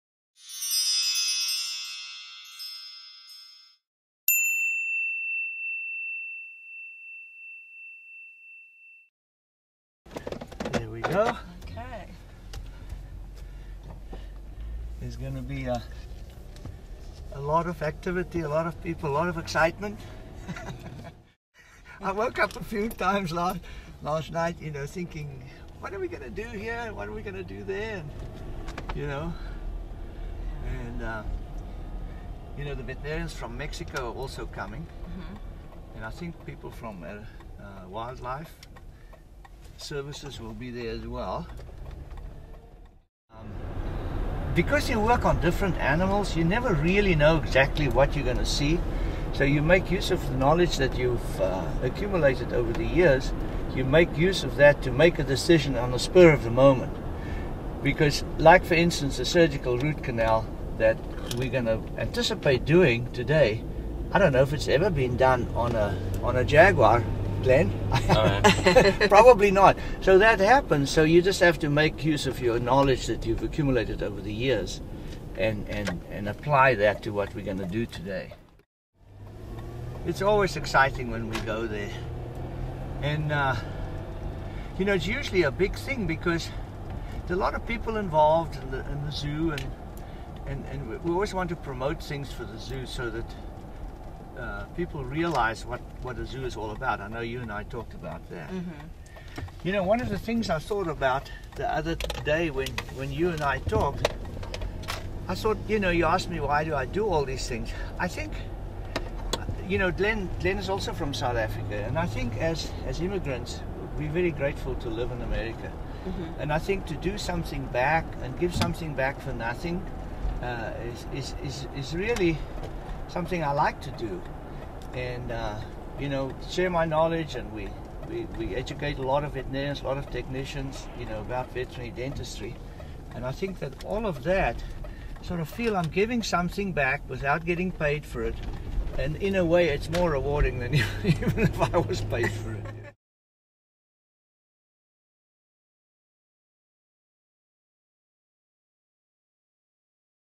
Lucero: The Veterinary Dentist
Follow our four-part series with Phoenix veterinary dentist Chris Visser, DVM, as he undertakes a root canal procedure on Lucero, a rare Sonoran jaguar who broke several teeth trying to chew his way through a rancher's trap in Mexico.

In this episode: Meet Visser as he drives to the Phoenix Zoo to perform the surgery. It's a first for him — and maybe a first for his profession!